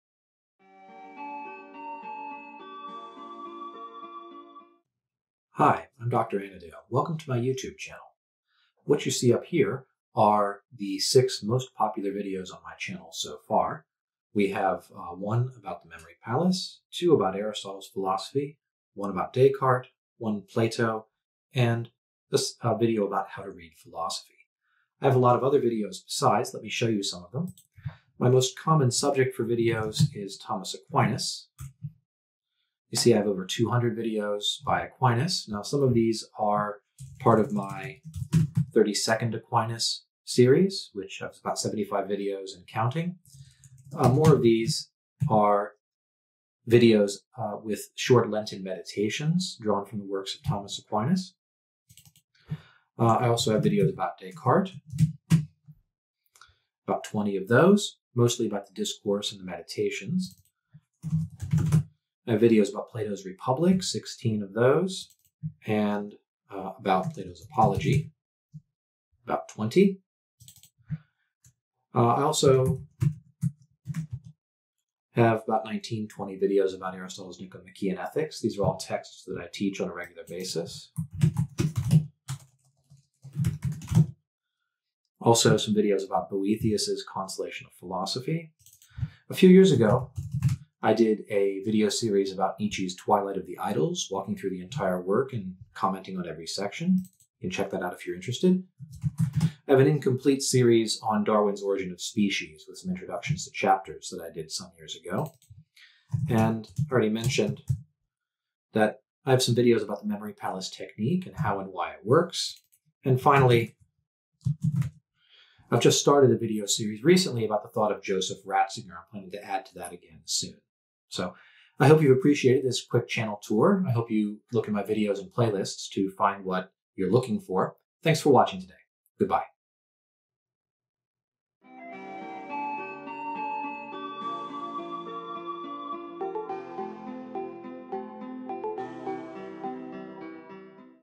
What My Videos Are About (April 2020)
A tour of my channel via the YouTube Studio search box

Thumbnail Image: The School of Athens, By Raphael - Stitched together from vatican.va, Public Domain,